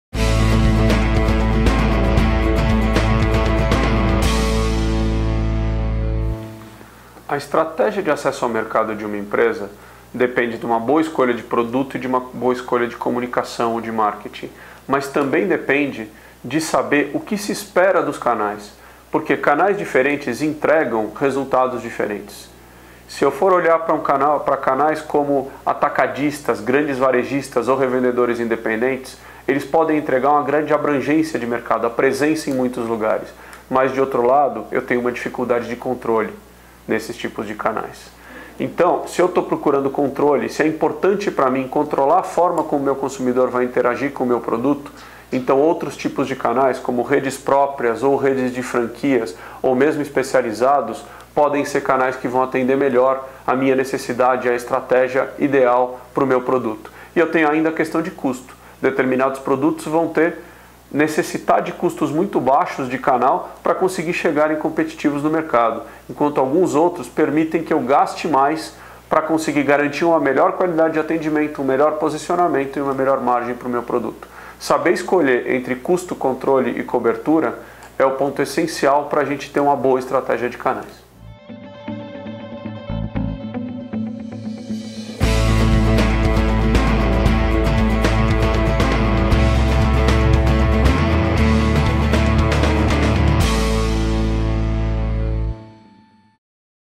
O que considerar ao montar a minha estratégia de acesso ao mercado?
Fernando Campora, sócio da Cherto Consultoria, explica que, ao definir uma estratégia de acesso ao mercado, a empresa deve saber exatamente o que espera dos canais de venda -- controle, custo ou cobertura?